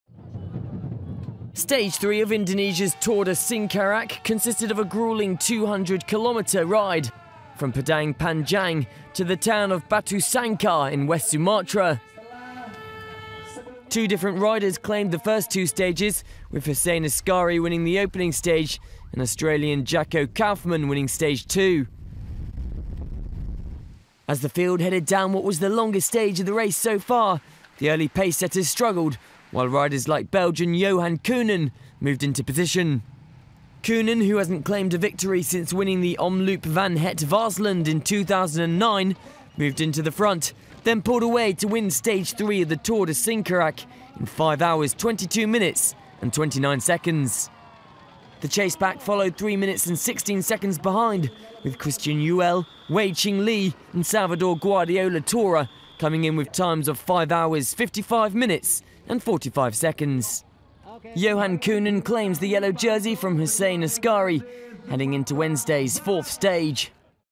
Tour de Singkarak - Johan Coenen takes lead after Stage 3 win
Belgian rider Johan Coenen has claimed the yellow leader's jersey after winning stage three of Tour de Singkarak.